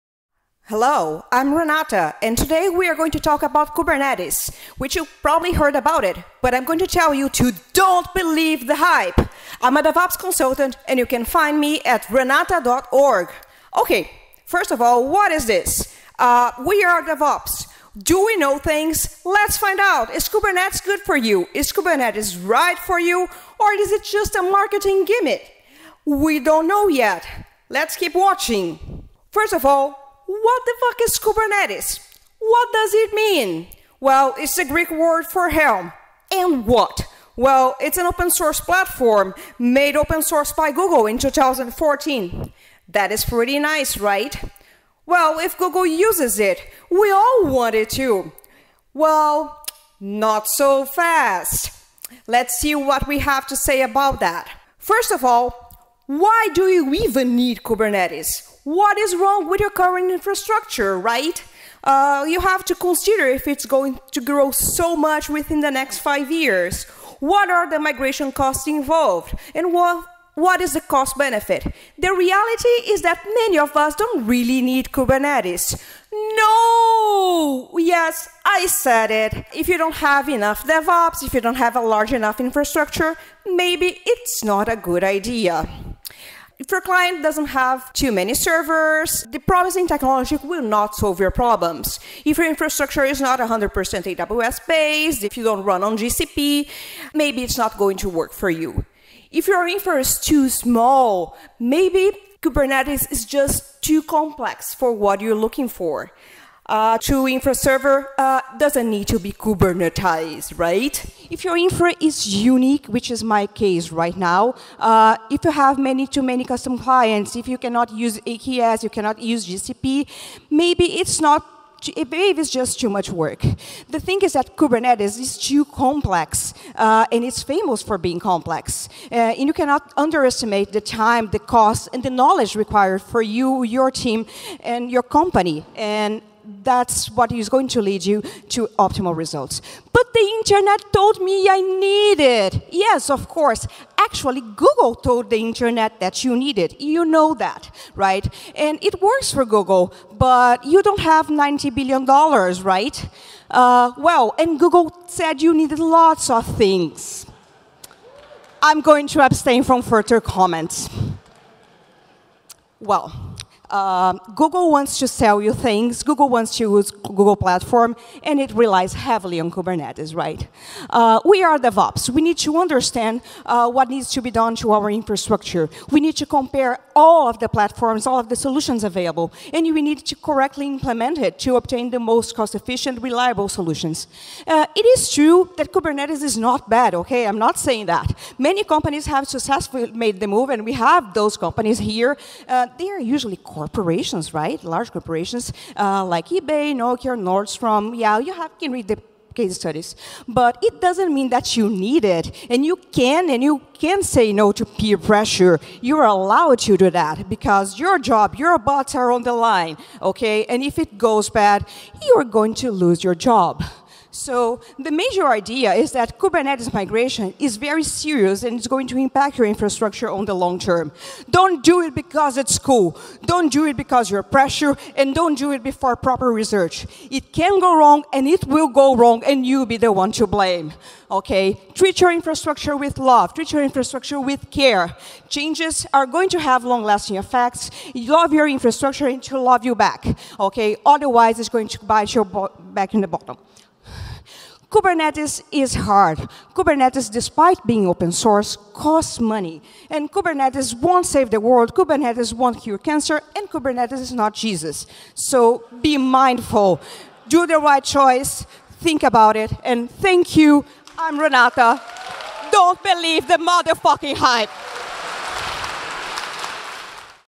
Renata Rocha, Kubernetes Hype - Do you really need it? - DevOpsDays Toronto 2019 Day 2 Ignite Talk
Video from Toronto's DevOps Days 2019 conference

Day 2 Ignite

Renata Rocha, DevOps Consultant - Sysadmin - Automation enthusiast - AWS addicted - Optimized for Cats!

K8S is everywhere today, and if you don’t LOVE it, you’re not cool. But does your infra really NEED it? A quick check on what K8S is, what it isn’t, what is it used for, why people love it and why it may not solve your problems at all!